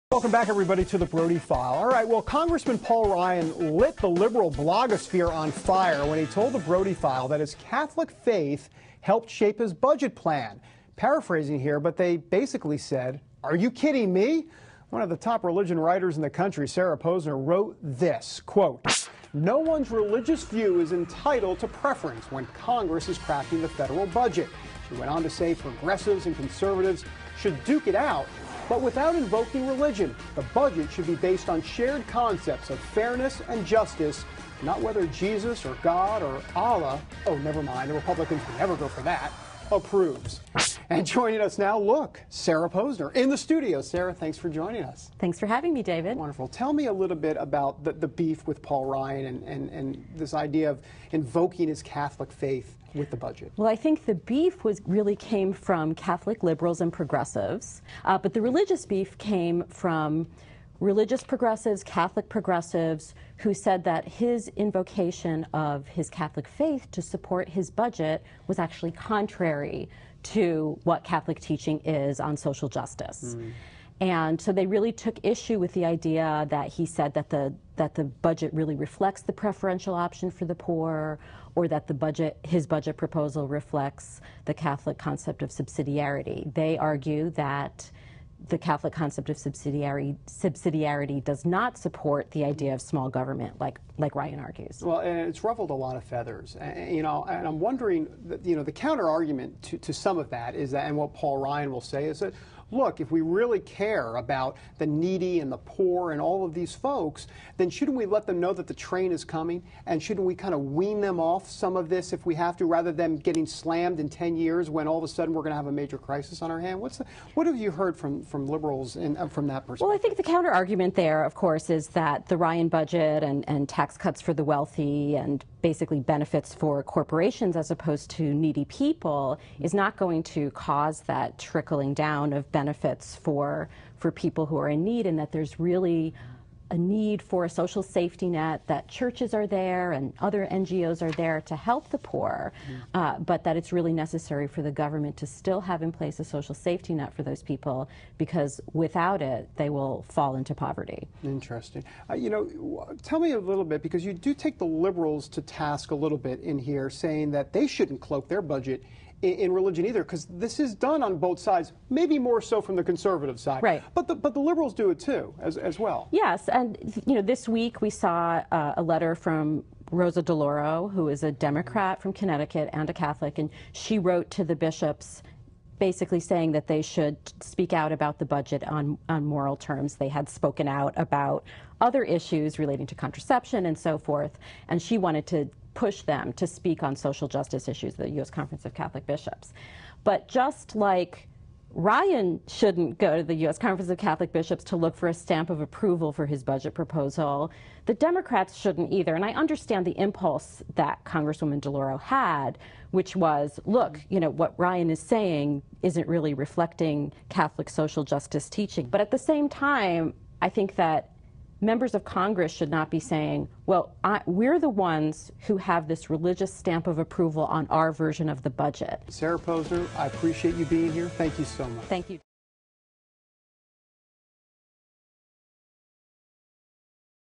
Sarah Posner: The Beef With Paul Ryan - CBN.com
Religion writer Sarah Posner has a bone to pick with Republican Paul Ryan -- who she says is using his Catholic faith to promote his budget plan... The Christian Broadcasting Network CBN